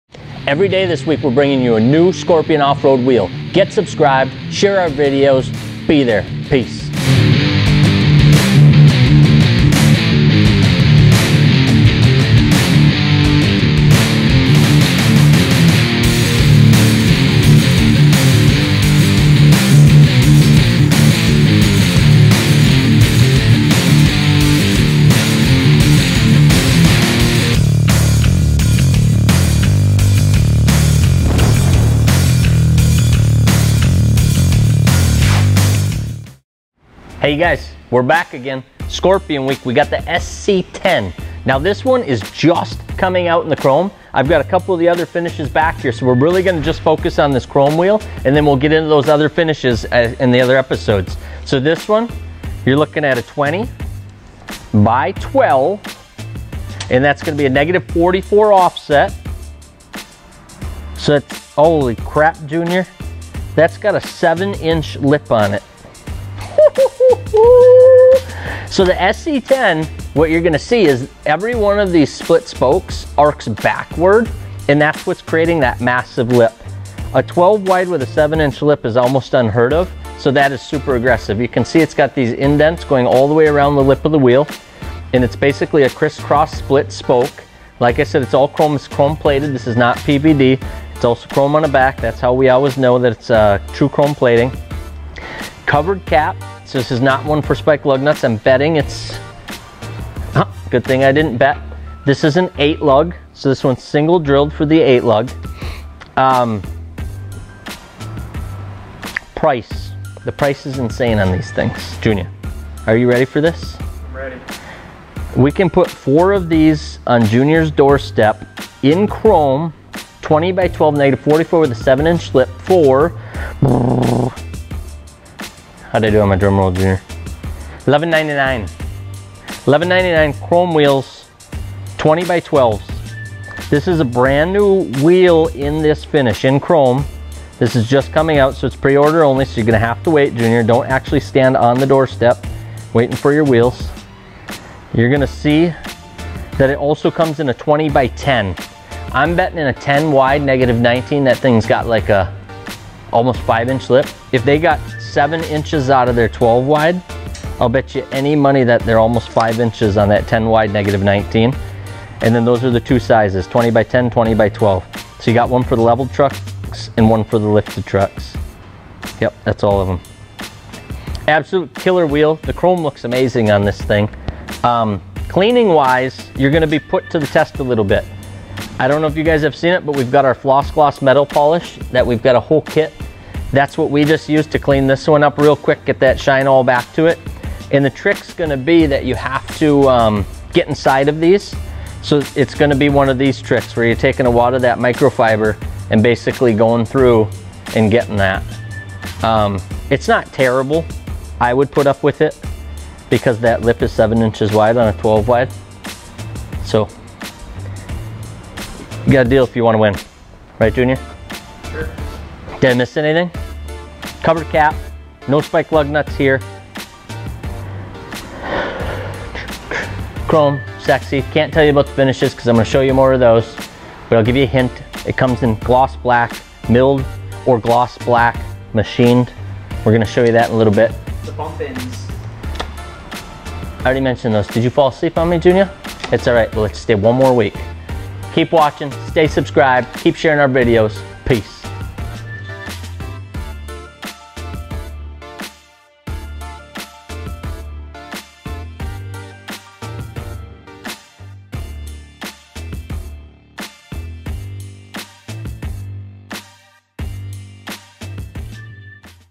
Scorpion Week Ep. 3 SC10 20x12 Chrome
Our third wheel of Scorpion Off-Road Wheels Week is the Scorpion SC10 in a Chrome 20x12 -44!